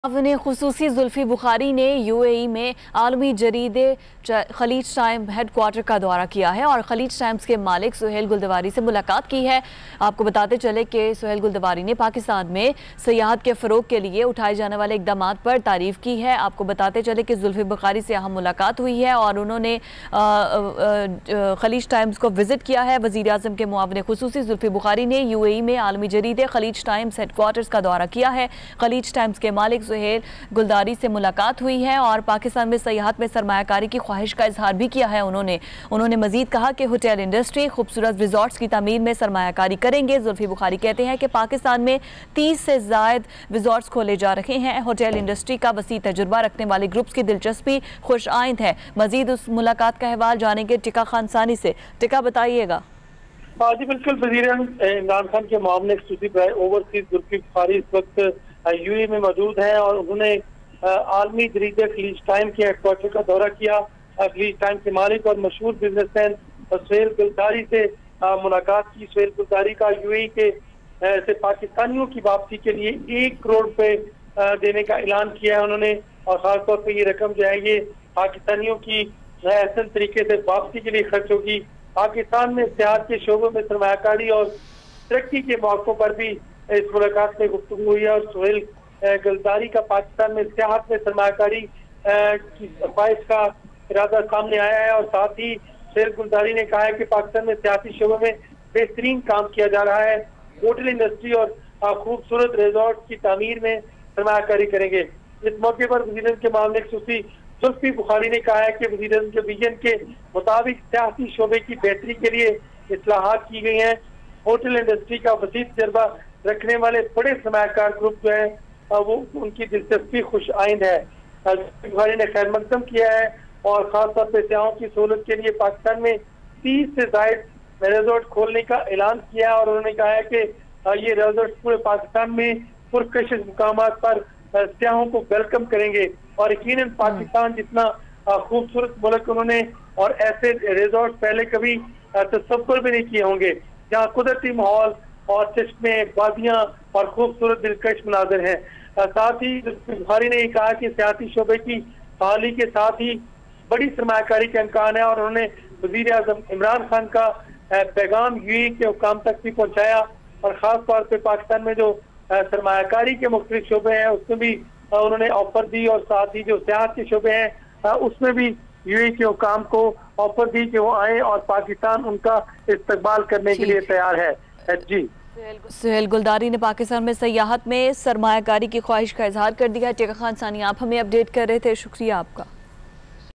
Suhail Galadari (Khaleej Times owner) expresses desire to invest in Pakistan
Suhail Galadari (Khaleej Times owner) expresses the desire to invest in Pakistan